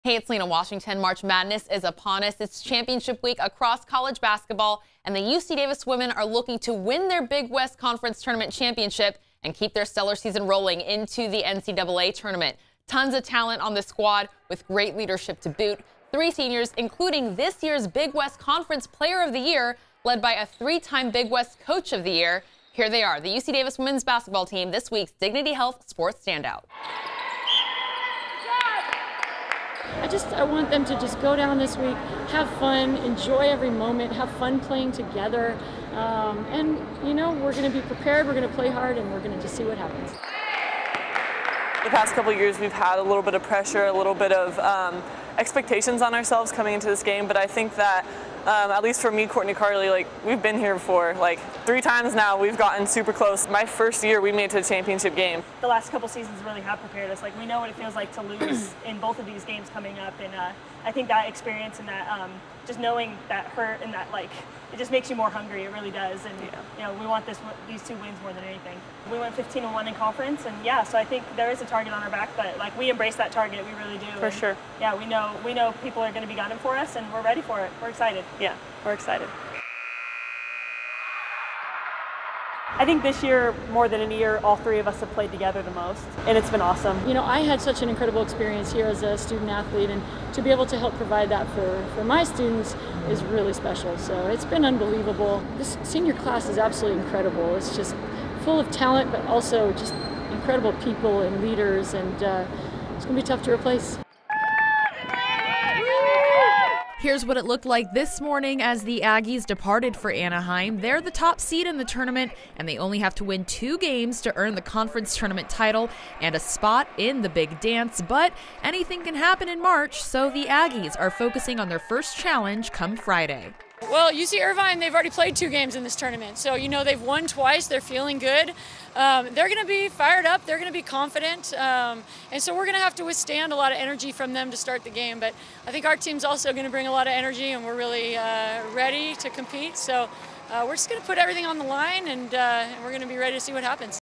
UC Davis Women's basketball team | Sports Standout
ABC10's Lina Washington features the UC Davis women's basketball team in this week's Sports Standout. Head Coach Jennifer Gross became only the second coach in league history to win three-straight coach of the year awards and the first since UC Santa Barbara's Mark French in 1996-98.